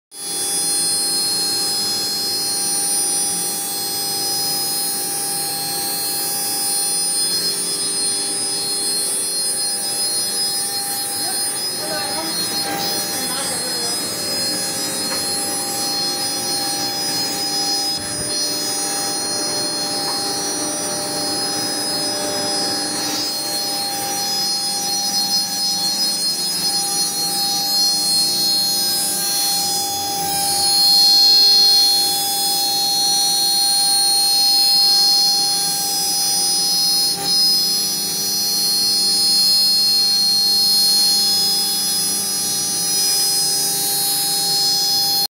Máquina de corte de madera con enrutador CNC K1-1325
Aplicable en distintos materiales:
Madera sólida, Madera dura y blanda, MDF, aglomerados, melamínicos, contrachapados.
Plásticos ABS, PVC, policarbonato, acrílico, sintra, polietileno, fenólicos, acetato, etc
Compuestos como Foam, EVA, goma espuma, caucho de silicon, fibras de vidrio, fibras de carbón, yeso, cuero y más.